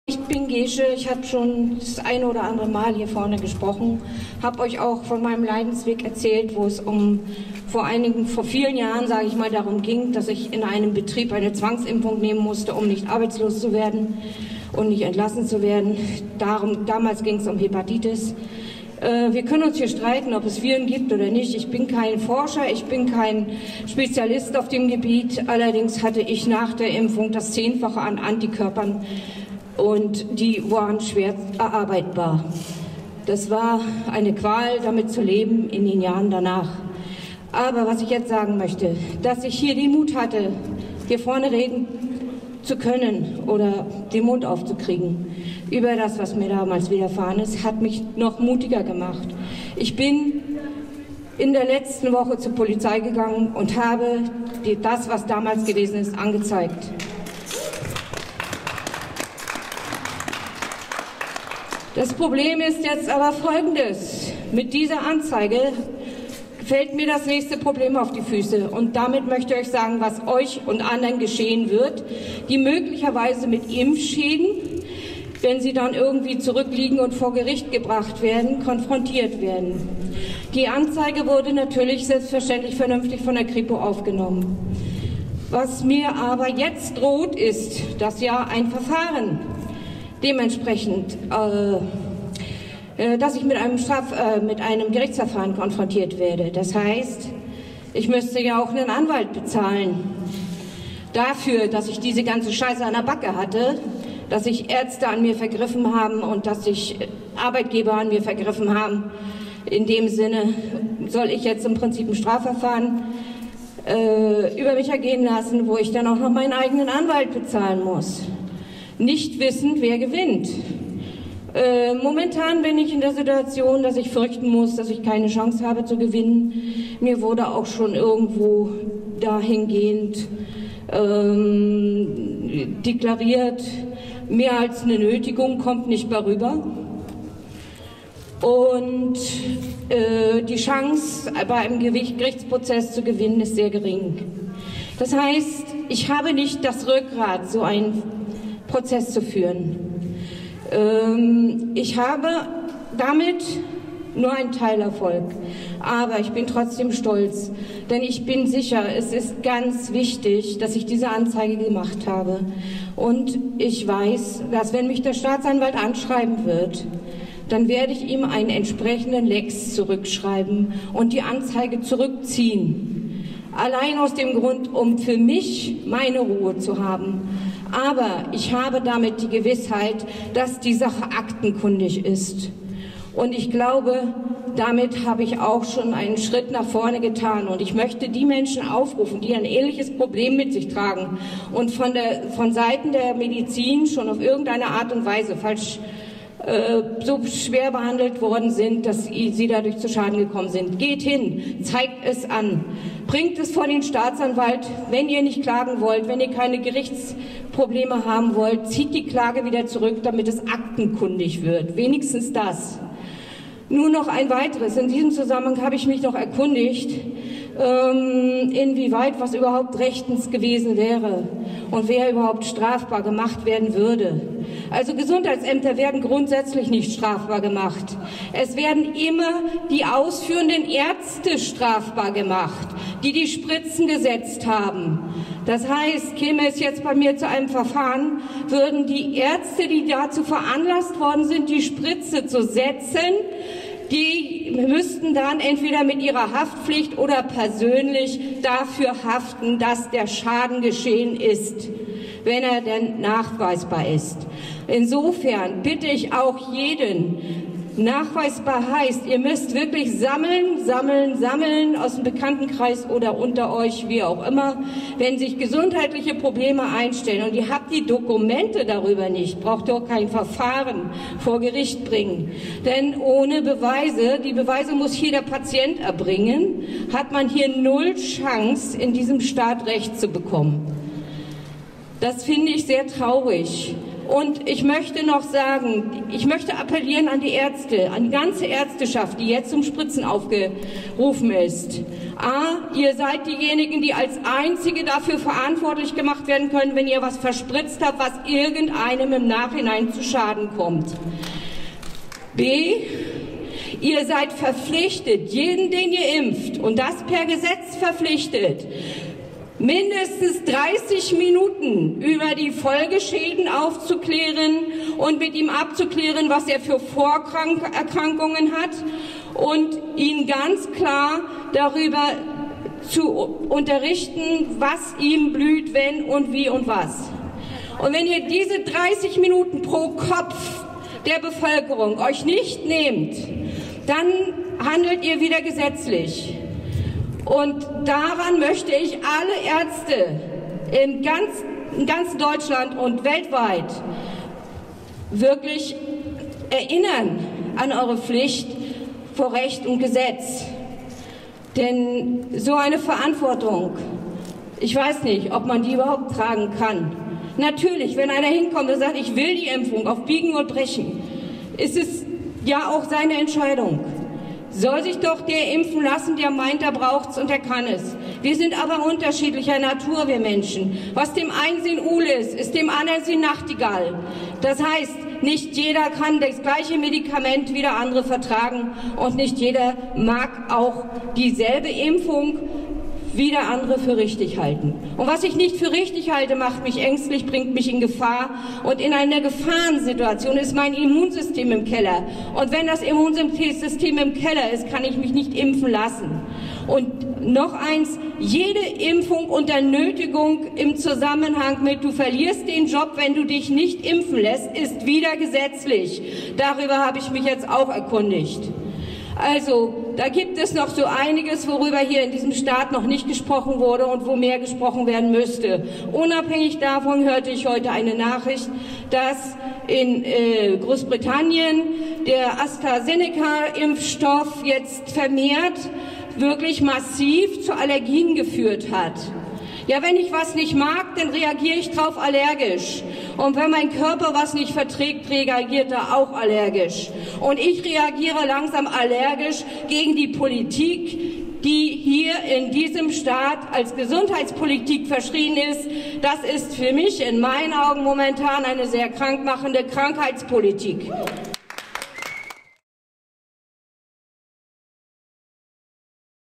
Gesche auf der Kundgebung in Stralsund
1Jahr Lockdown es Reicht.
Gesches Rede auf der Kundgebung am 12. März 2021 in Stralsund